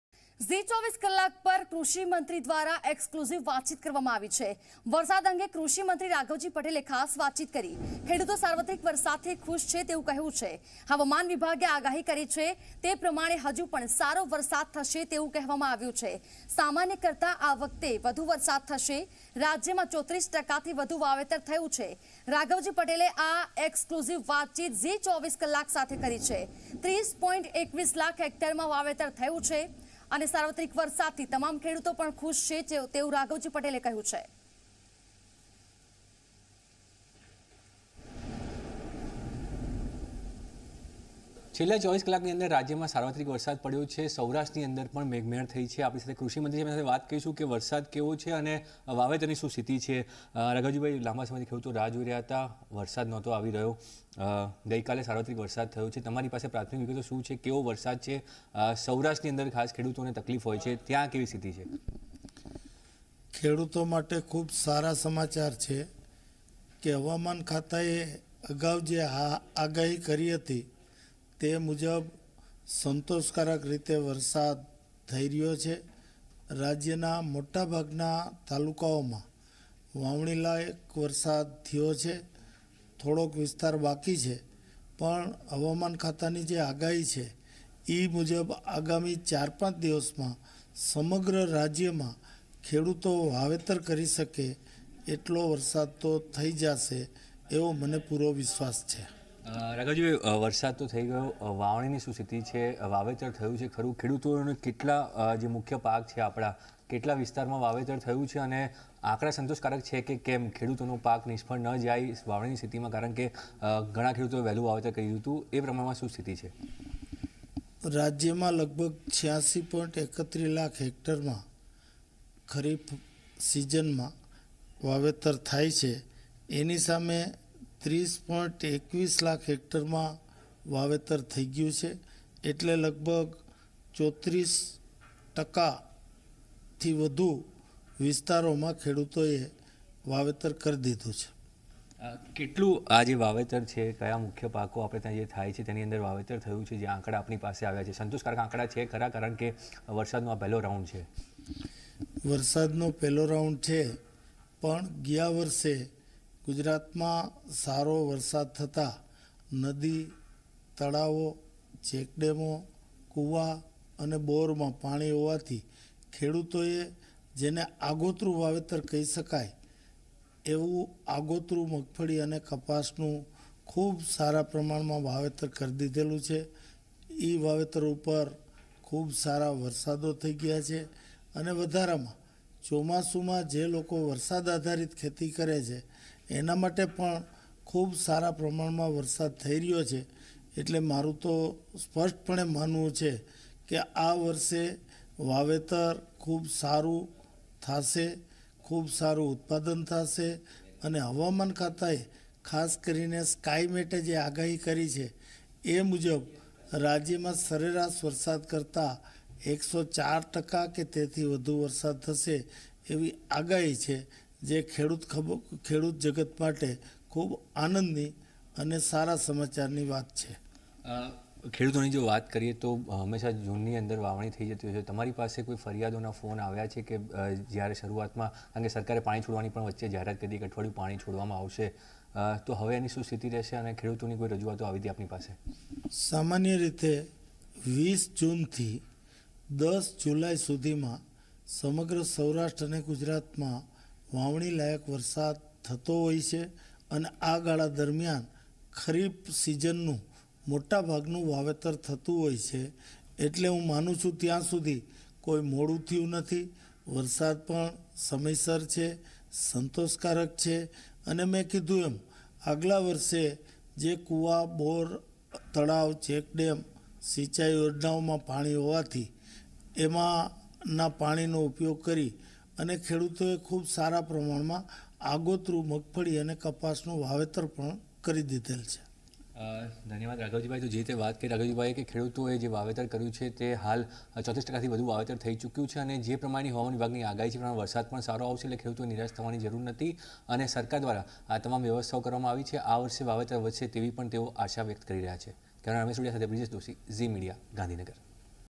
રાજ્યમાં 34 ટકાથી વધુ વાવેતર થયું, આગામી દિવસોમાં વધુ સારા વરસાદની હવામાન વિભાગની આગાહી:રાઘવજી પટેલ
રાજ્યમાં 34 ટકાથી વધુ વાવેતર થયું, આગામી દિવસોમાં વધુ સારા વરસાદની હવામાન વિભાગની આગાહી: કૃષિ મંત્રી રાઘવજી પટેલ